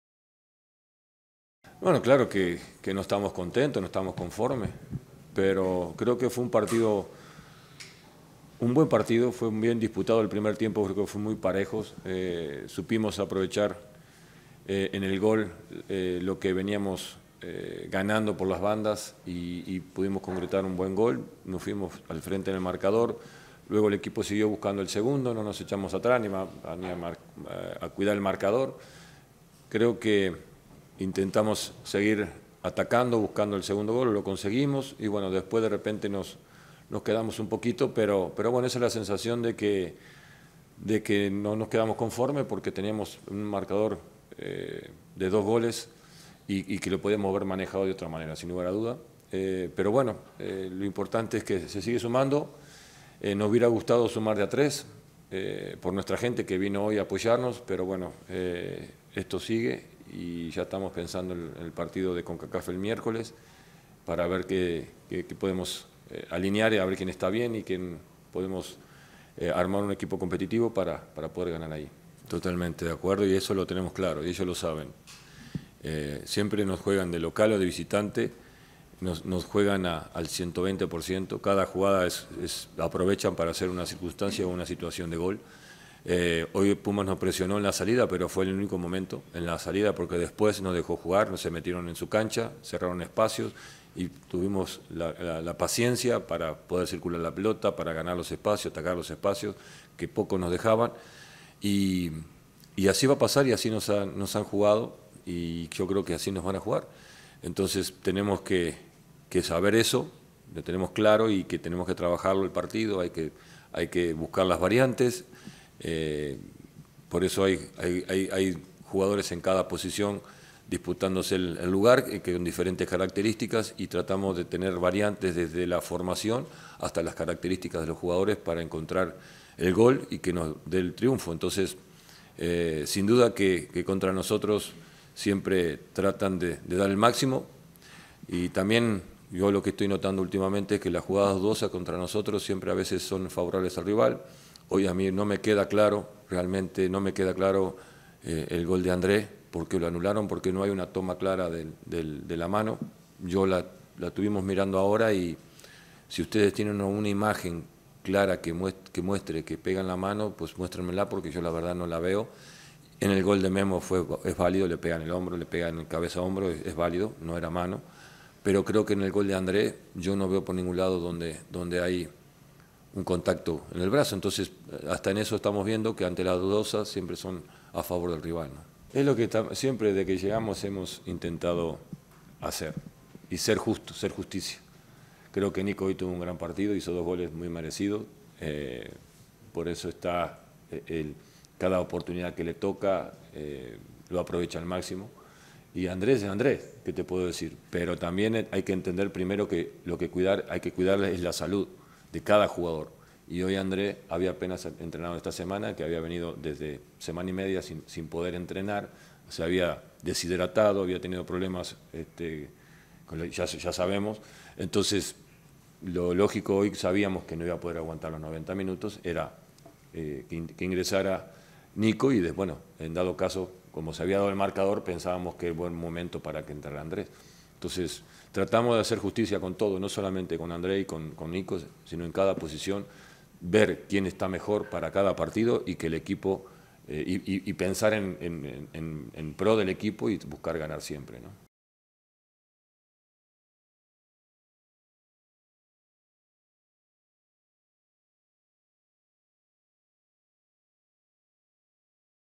Siboldi: “No hay que echar culpas a nadie. Aquí ganamos todos y perdemos todos”
Conferencia de prensa con el D.T. del Club Tigres de la U.A.N.L tras su empate como local ante su similar de los Pumas de la UNAM, partido correspondiente a la jornada 5 de la Liga MX, Para más información visita